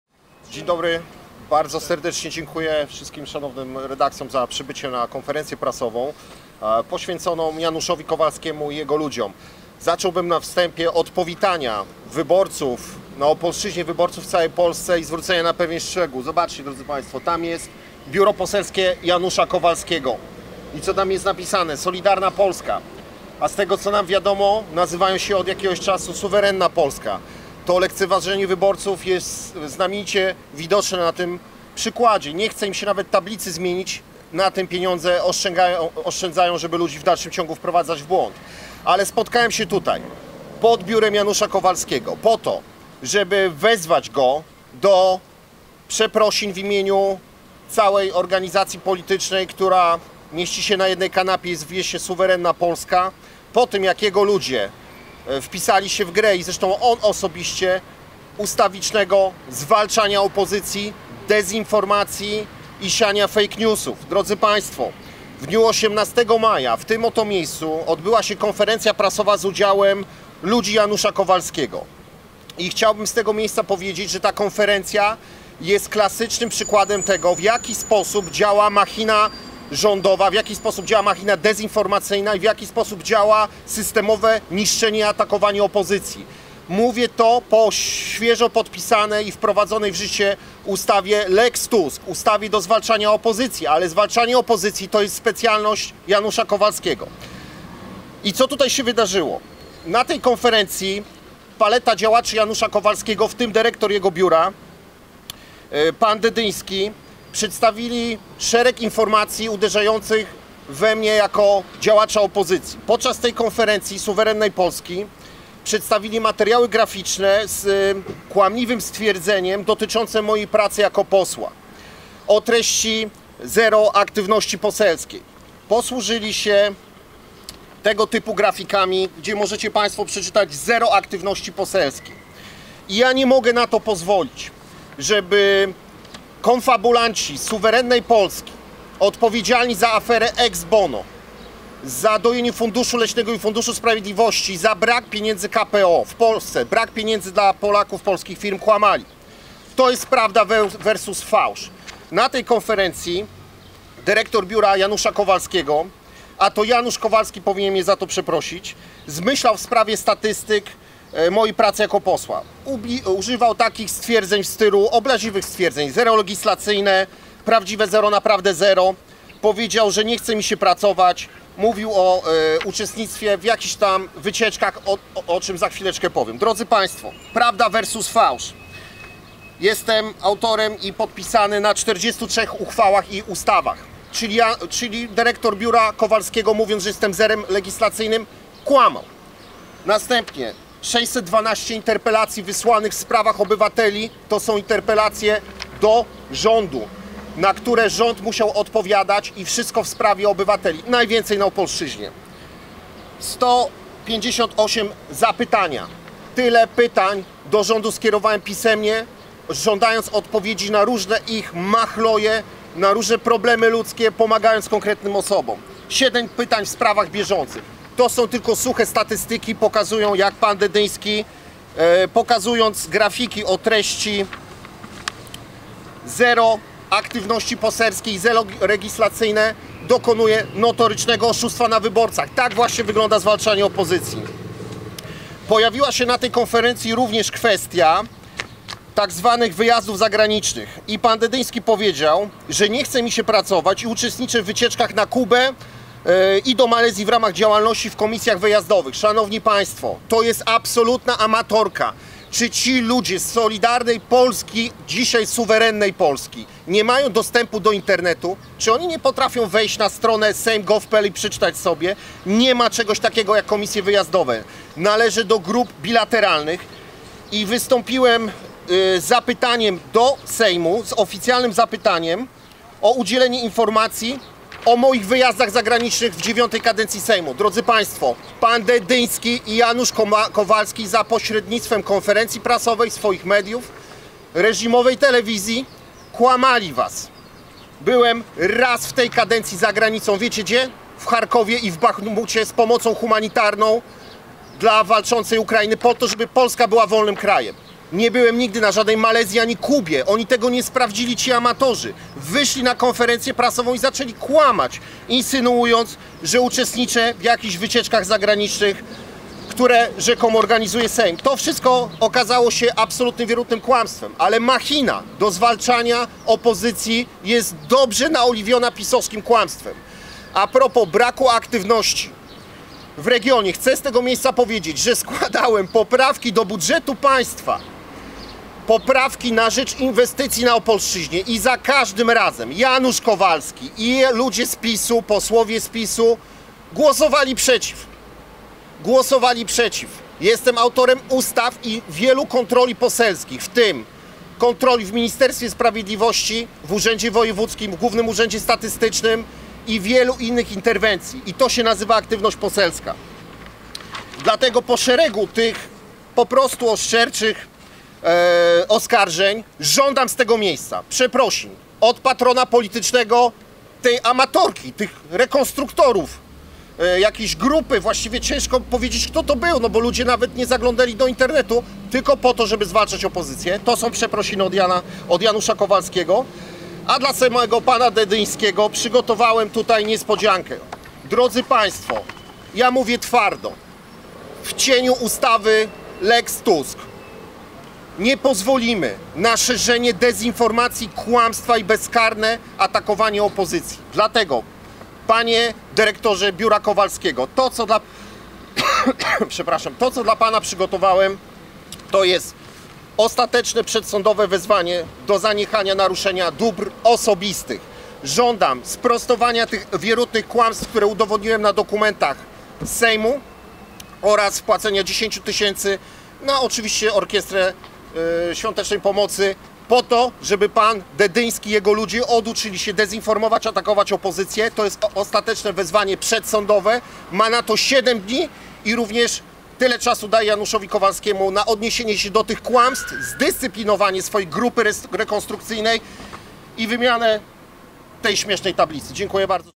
Konferencja prasowa posła Nowoczesnej, Witolda Zembaczyńskiego
Żądam przeprosin ze strony posła Janusza Kowalskiego, a do dyrektora jego biura, pana Dydyńskiego, wysyłam pismo przedsądowe z żądaniem sprostowania kłamstw na mój temat, a także zadośćuczynienia w postaci wpłaty 10 tysięcy złotych na konto WOŚP – stwierdził na konferencji prasowej przed biurem poselskim Janusza Kowalskiego w Opolu poseł Nowoczesnej Witold Zembaczyński.